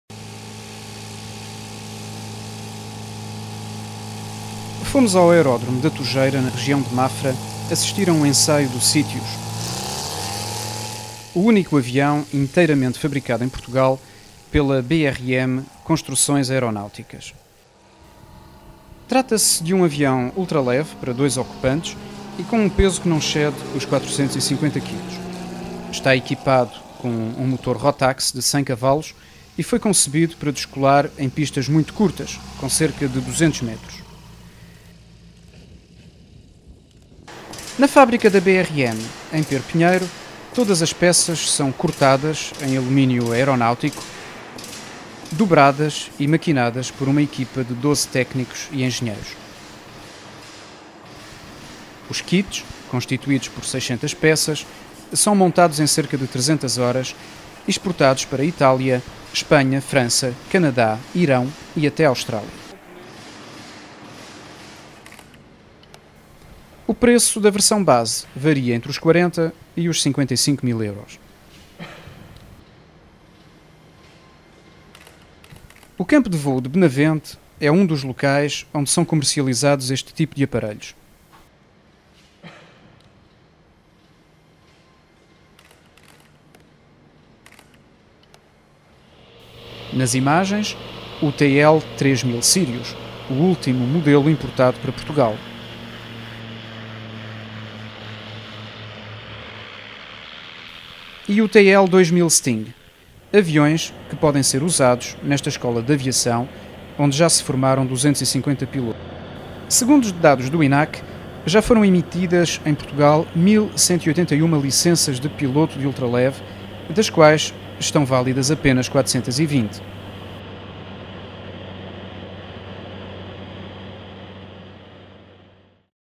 Único avião produzido em Portugal é ultraleve
O segmento dos ultraleves é a mais popular forma de voar em Portugal. Até à data, já foram emitidas 1.181 licenças de piloto de ultraleve pelo INAC - Instituto Nacional de Aviação Civil (das quais estarão válidas apenas 420) e encontram-se registadas 385 aeronaves deste tipo no Registo Aeronáutico Nacional.